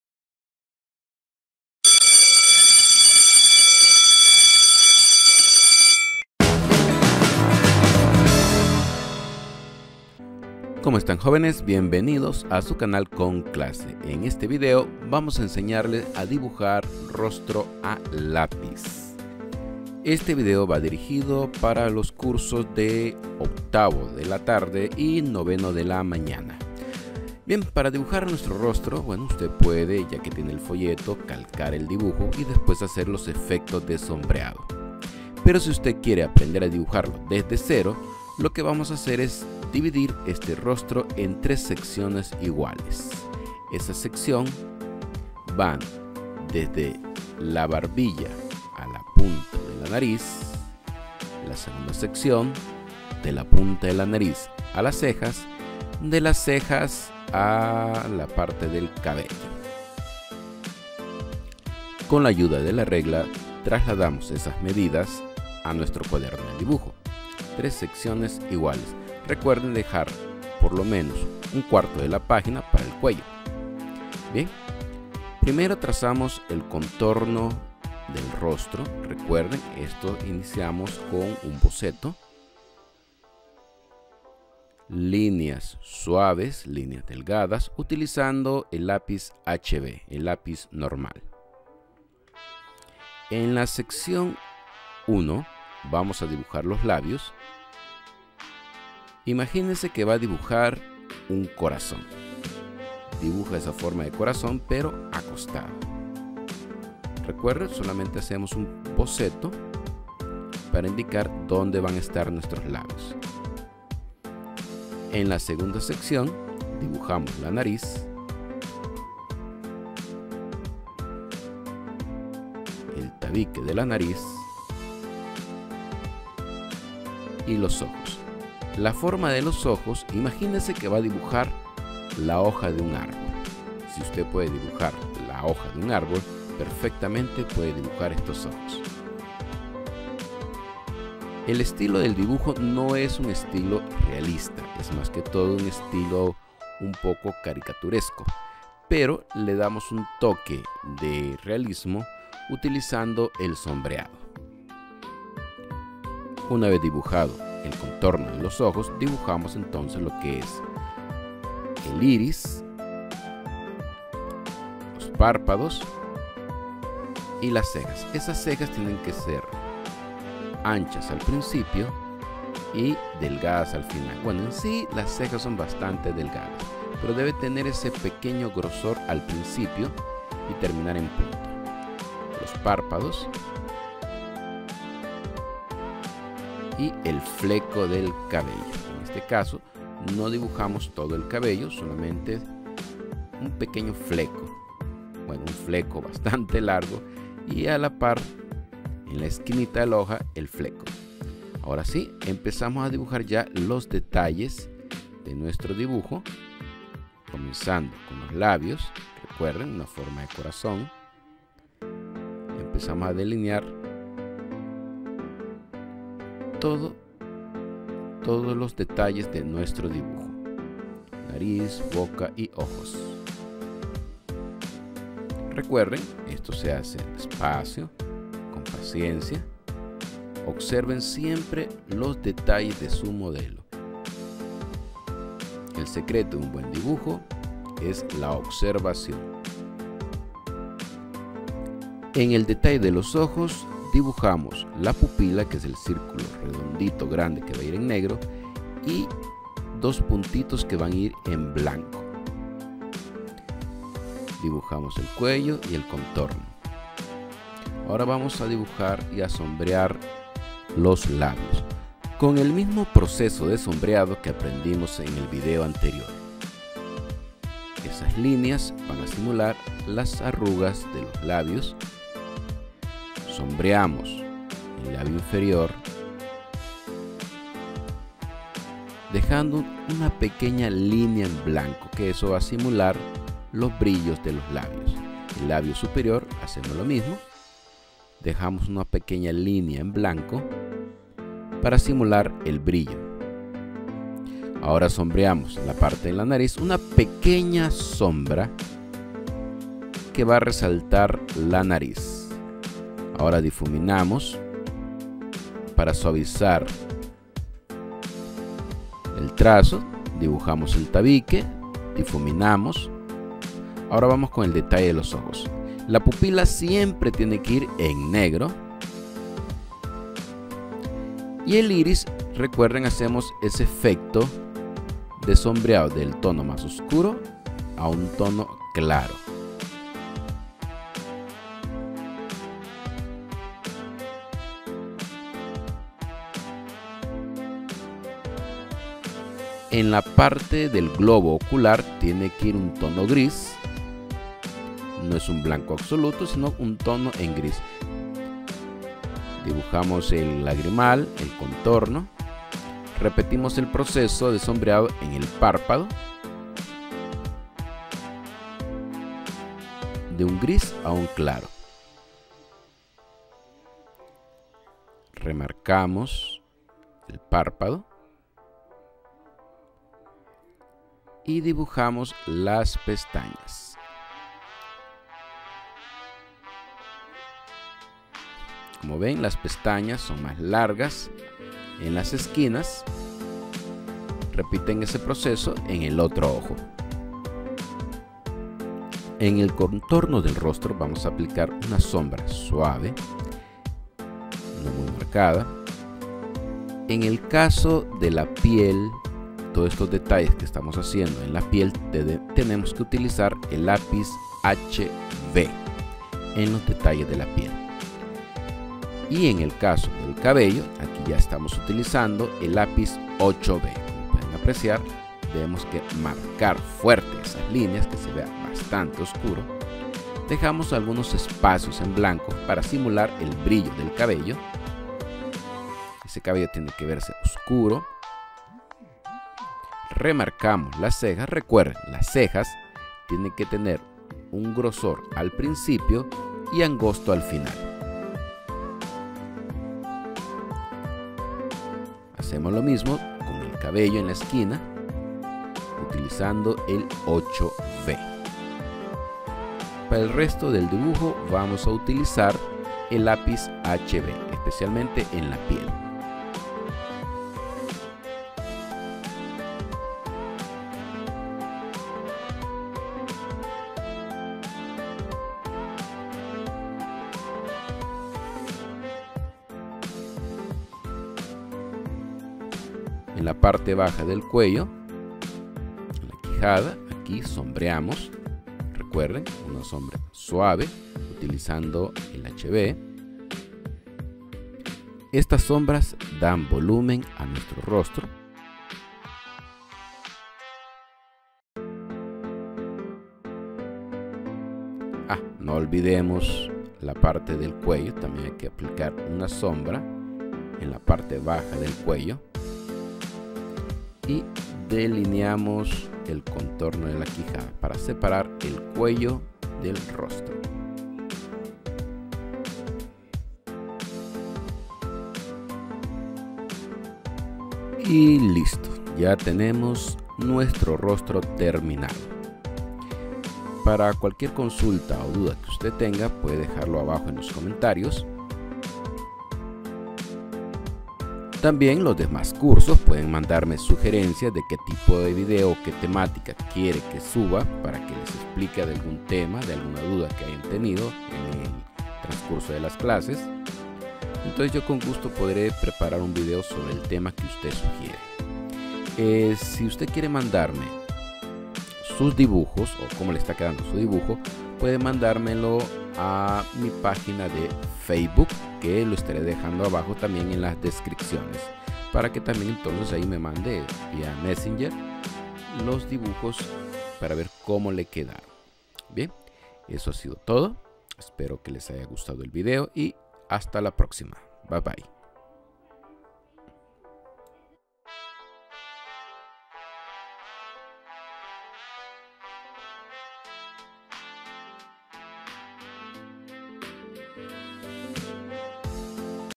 ROSTRO A LAPIZ
DIRIGIDO 8vo GRADO DE LA TARDE Y 9no DE LA MAÑANA, aprenderemos a dibujar un rostro a lápiz. si desean mostrar sus trabajos pueden enviarlo a los grupos de whatsapp o mensaje a mi pagina de facebook,